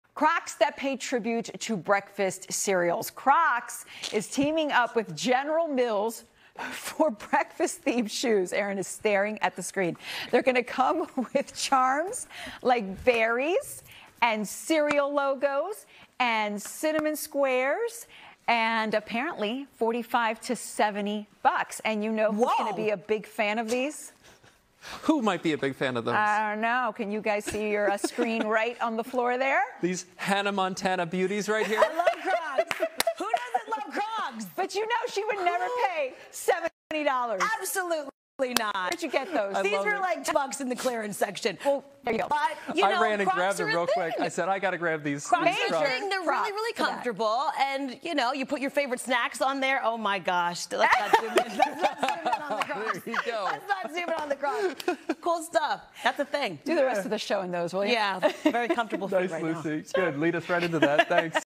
Breakfast Cereal Crocs Are Really Becoming a Thing
NBC10’s Lucy Bustamante has the details on General Mills teaming up with Crocs for a new line of colorful footwear. Will Crocs-wearer Erin Coleman buy a pair?